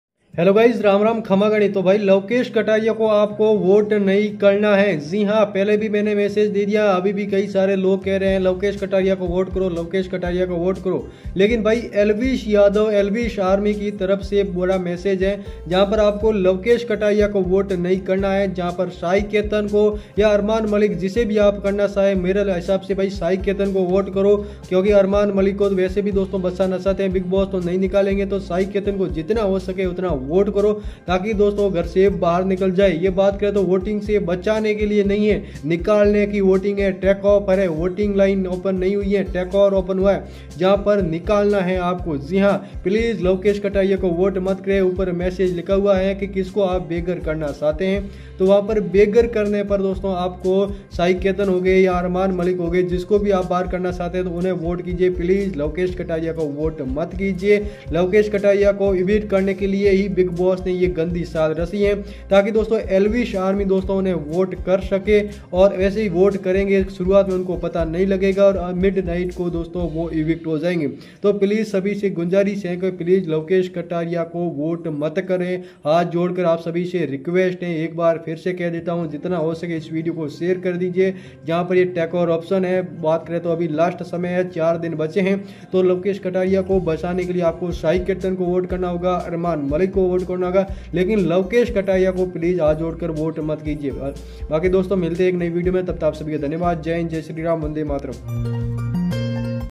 Please Elvish Army Lovekesh Kataria को Vote मत करो, Elvish Yadav Important Message Bigg Boss OTT 3
This video is only made for reaction and review purpose and to give my opinion

Thankyou